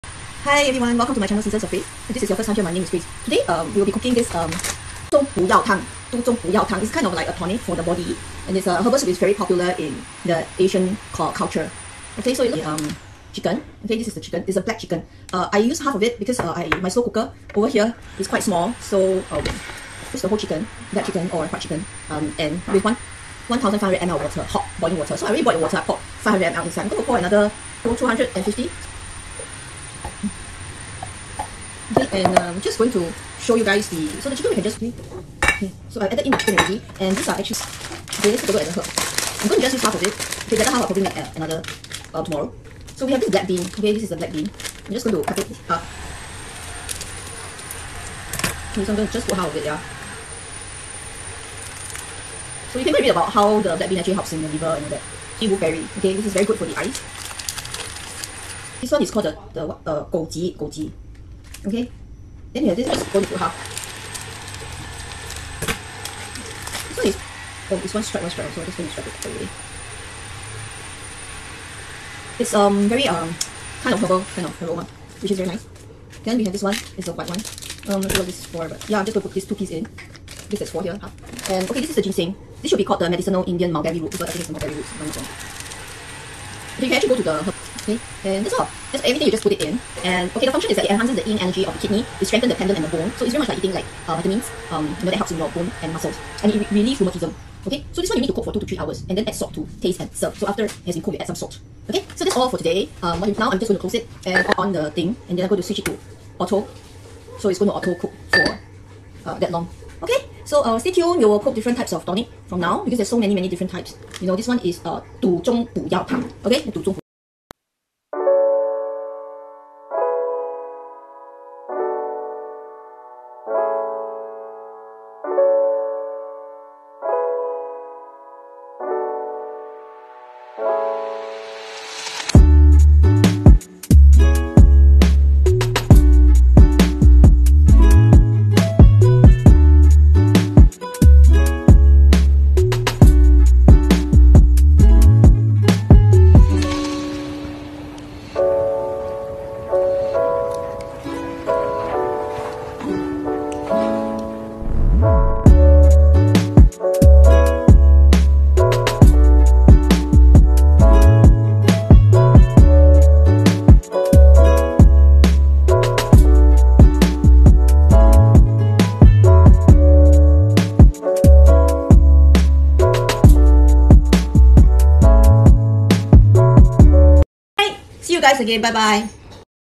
Herbal soup cooking Du Zhong Bu Yao Tang 杜.仲补腰湯
WFH, HPL & P2HA P3HA.. P2HA .. P3 ?

Du Zhong Bu Yao Tang
杜.仲补腰湯

Vitamins versus Chinese medicinal herbs .
I love drinking these tonic soup and like the taste of herbs . ￼

Who likes tonic soup ?